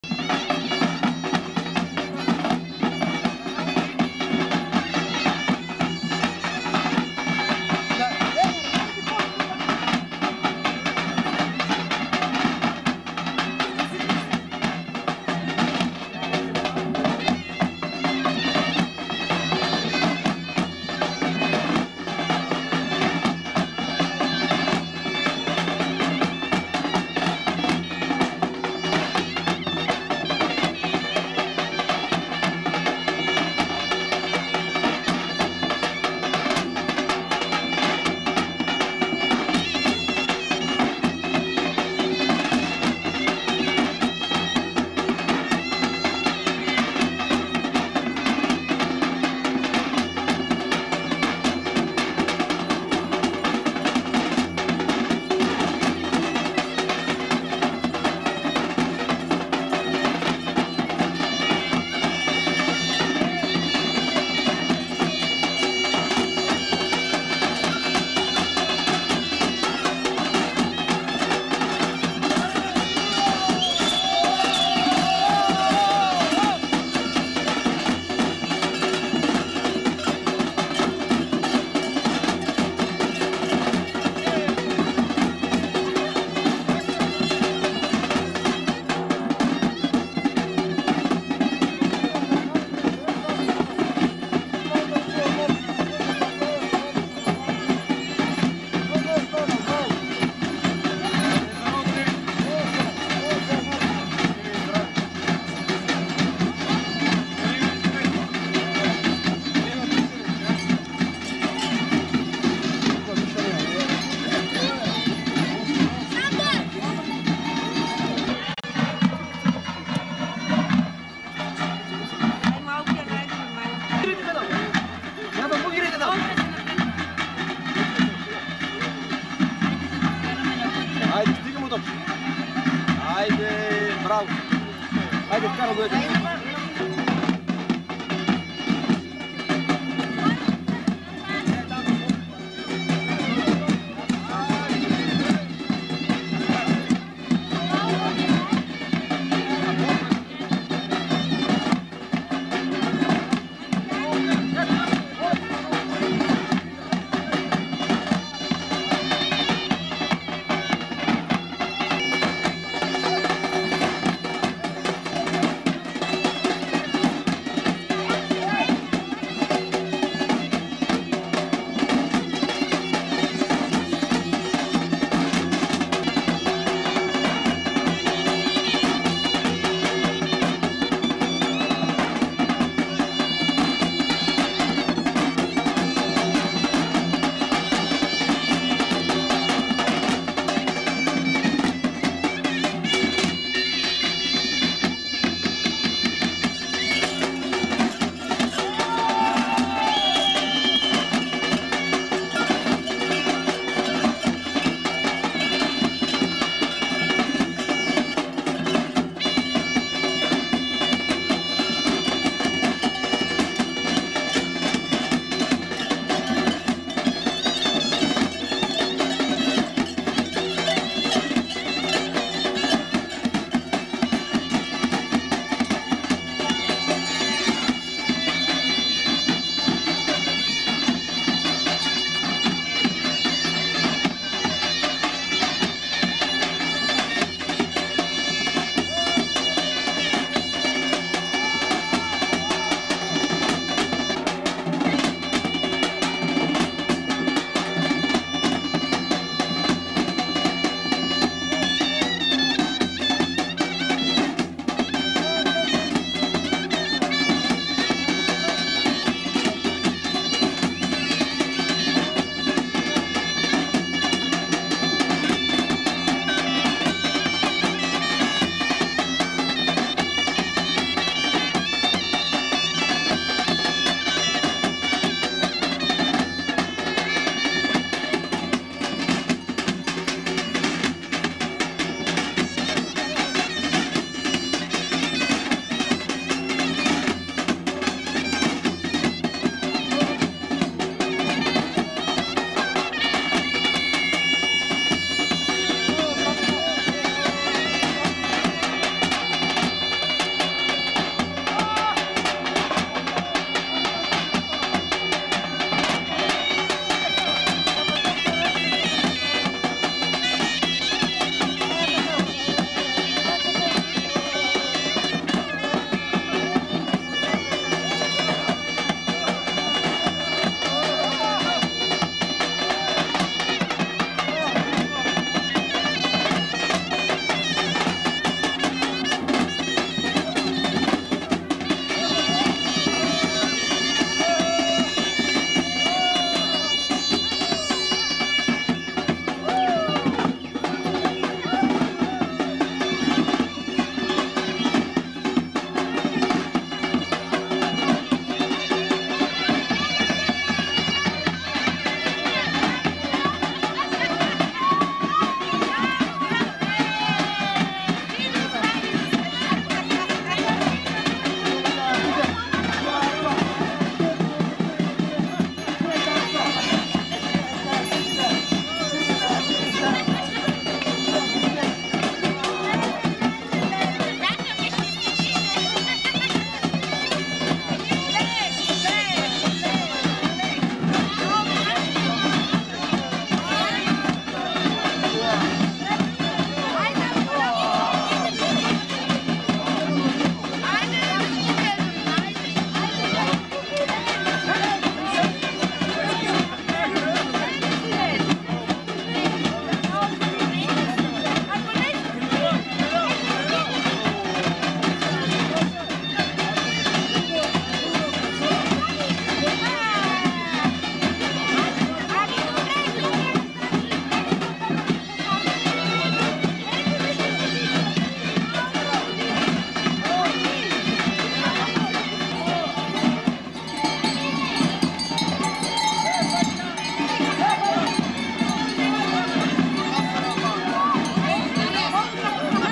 Сурва 2012 в село Железница, община Симитли, област Благоевград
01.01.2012 - Нова година и Василовден в село Железница.